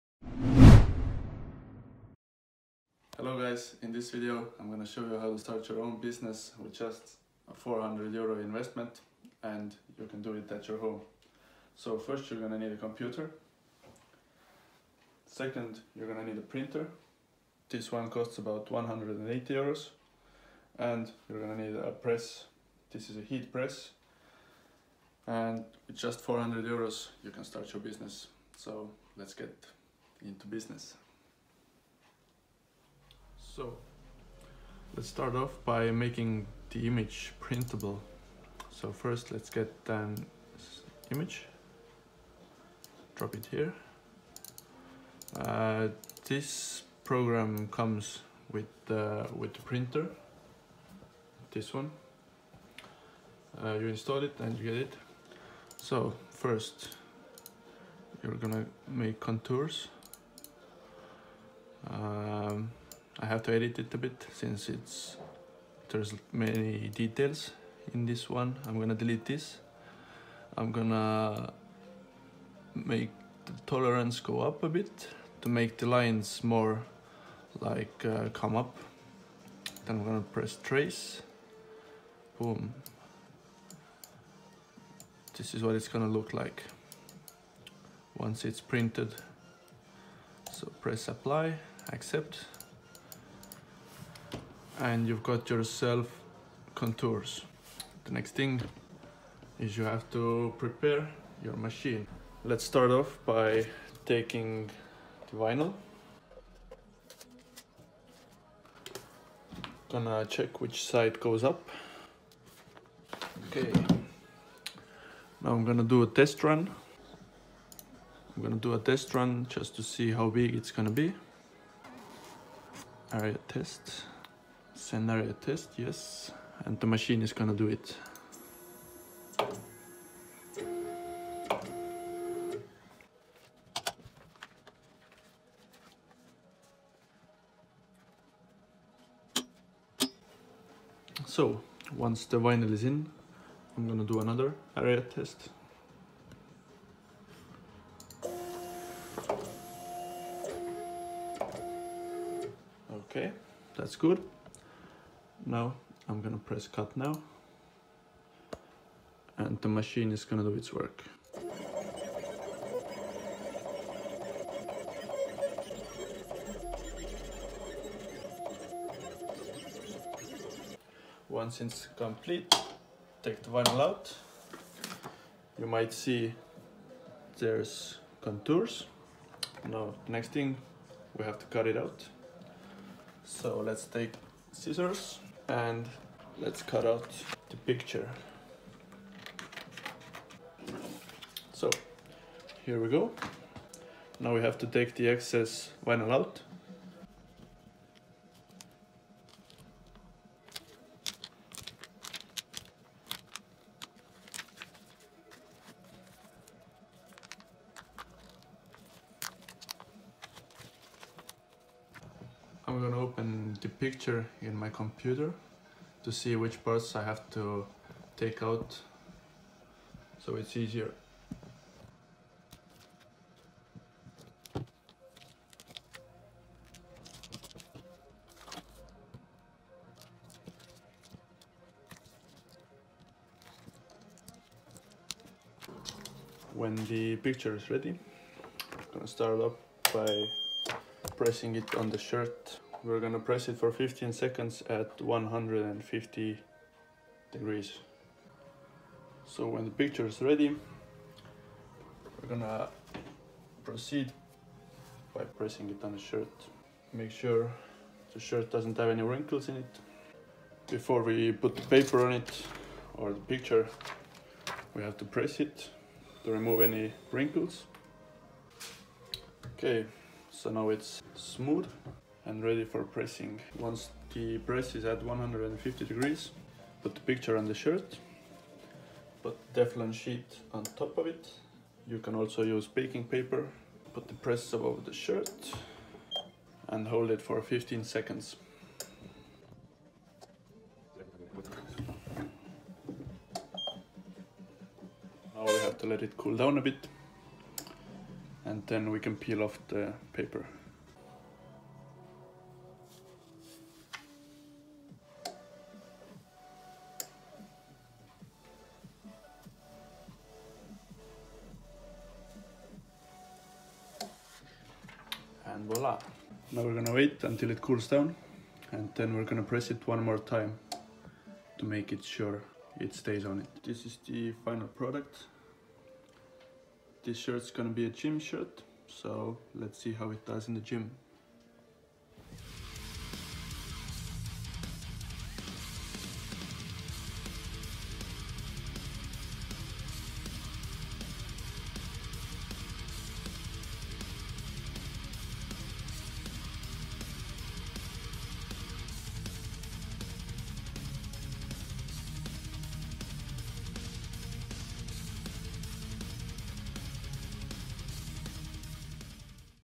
T-SHIRT Printing HOME Business - How To Earn Extra Money From Home
A quick overview of machines and techniques used to print t-shirts.